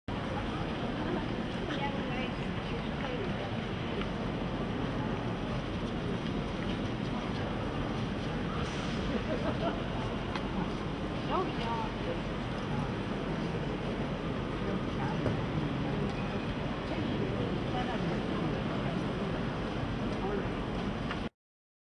International Film & Video
A rose window and arches accent the inside of Notre Dame Cathedral in Paris, France.